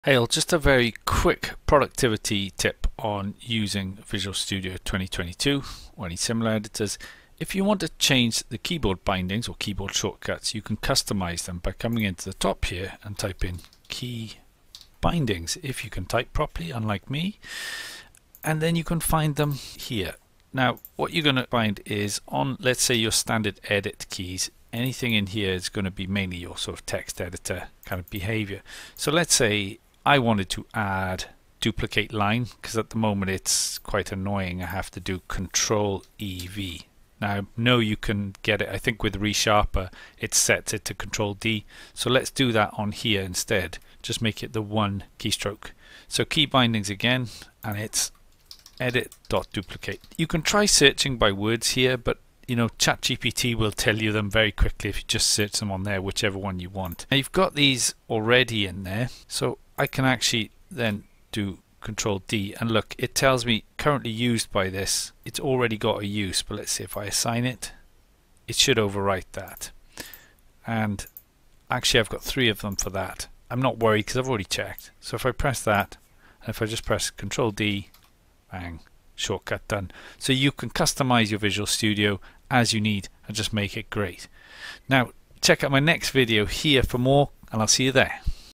Code FASTER - How to Customise Keyboard Shortcuts in Visual Studio 2022
Remember - it's never too late!

Cheers,
Steve